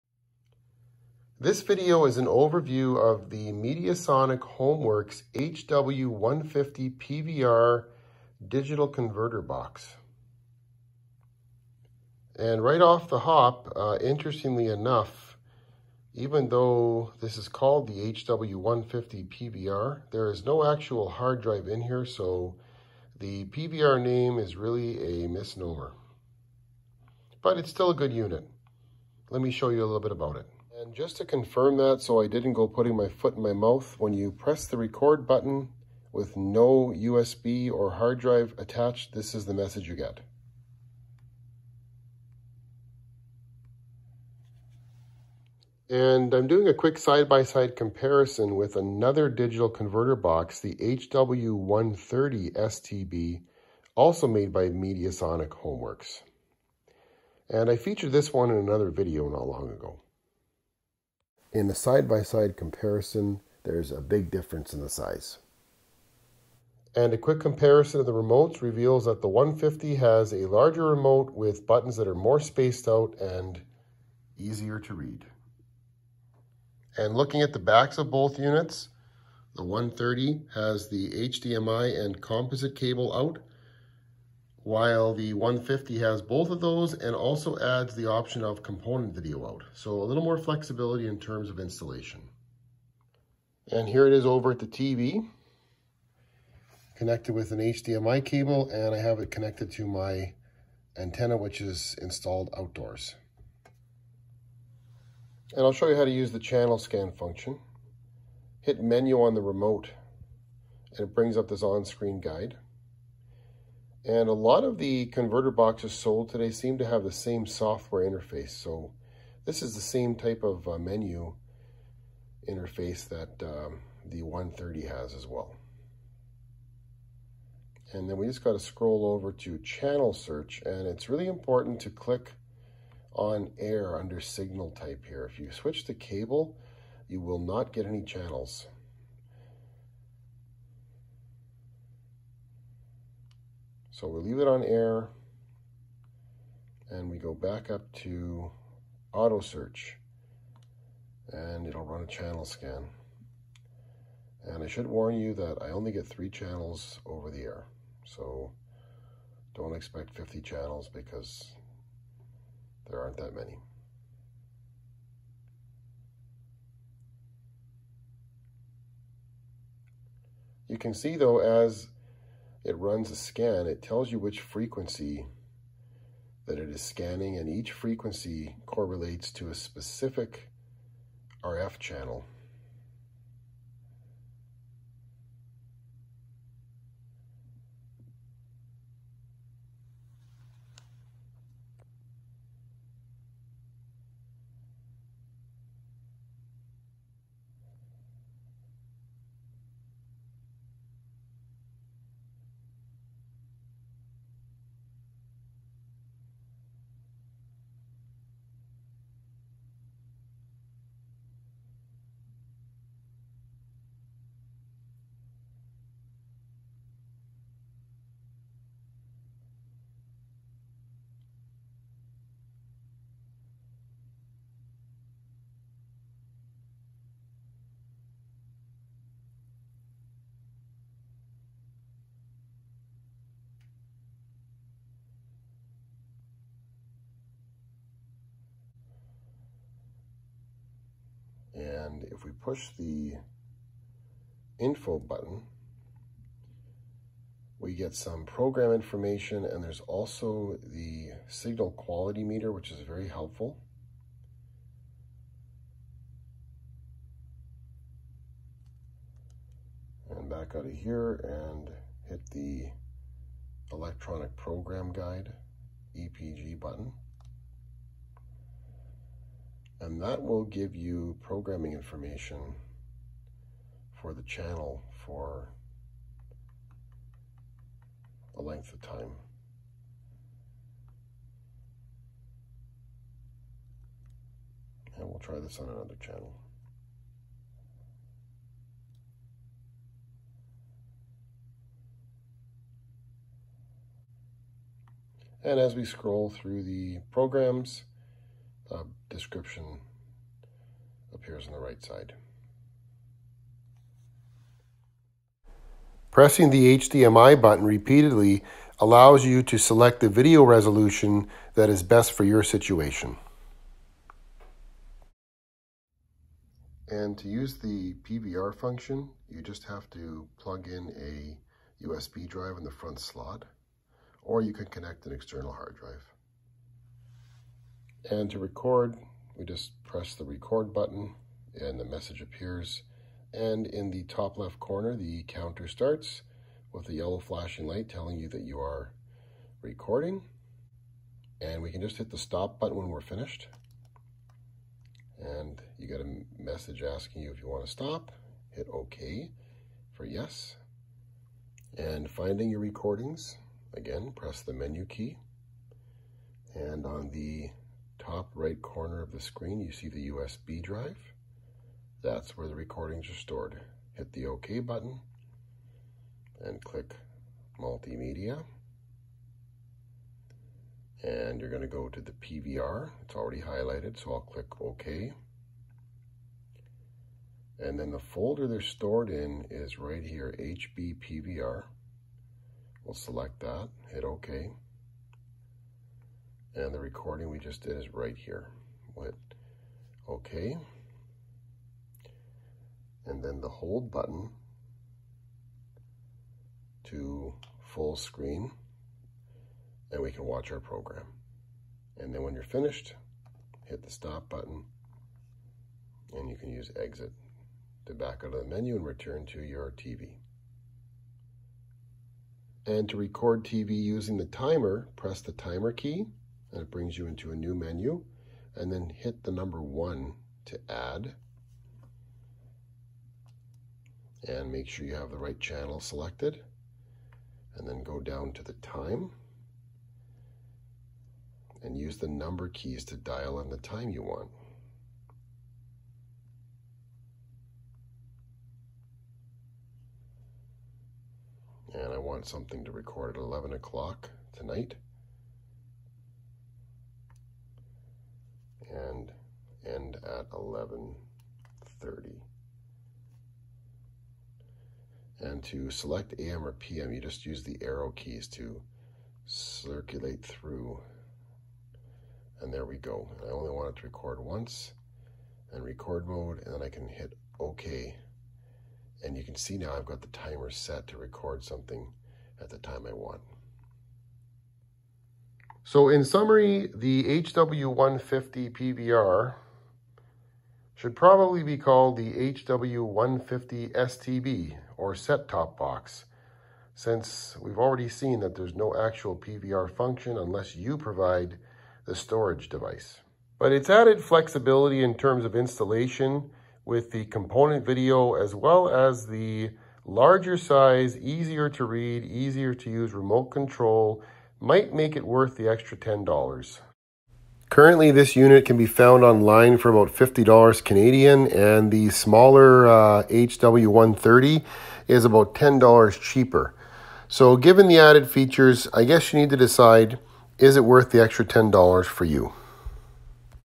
Mediasonic Homeworx HW150PVR digital converter box Overview | Free over the air antenna TV Recorder
A look at and how to use the Mediasonic Homeworx HW150PVR digital converter box Overview | Free over the air antenna TV. Overview of the features and functions.
A great unit for cord cutters who want to receive free over the air television with a TV antenna.